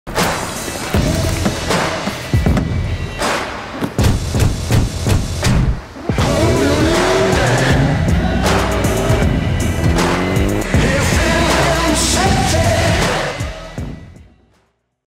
FIVE FOR FREEDOM. THE MINI 5-DOOR.
The MINI 5-Door puts a spacious spin on the original icon without compromising its renowned go-kart driving experience. Starting from TT$285,000.